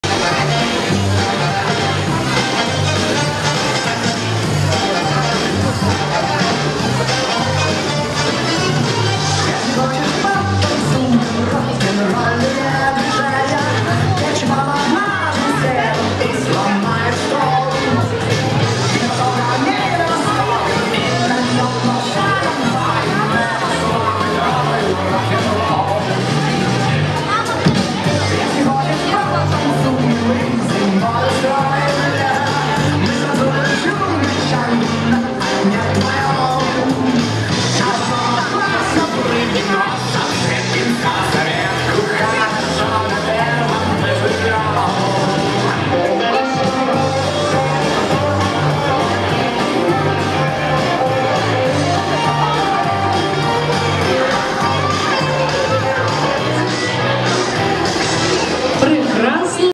BALLROOM DANCING/БАЛЬНЫЕ ТАНЦЫ.квик степ.'N'класс.финал.кубок мэра.Вышгород.16 04 2016.
Ювеналы 1 - 2 соло.